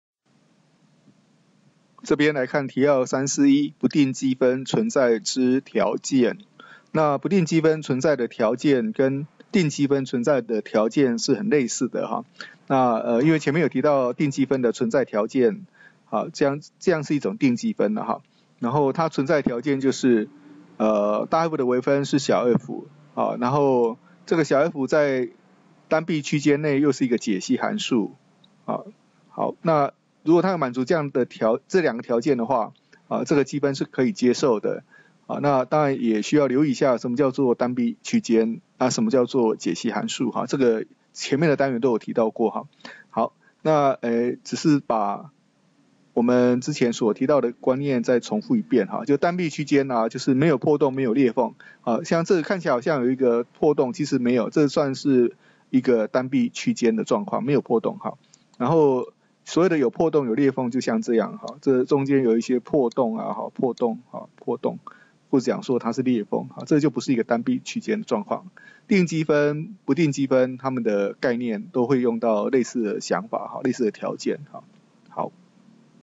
【教學影片】提要341：不定積分存在之條件▕ 授課教師：中華大學土木系呂志宗特聘教授
不定積分之存在性定理之前已有提過類似的觀念，本單元擬特別加強說明之。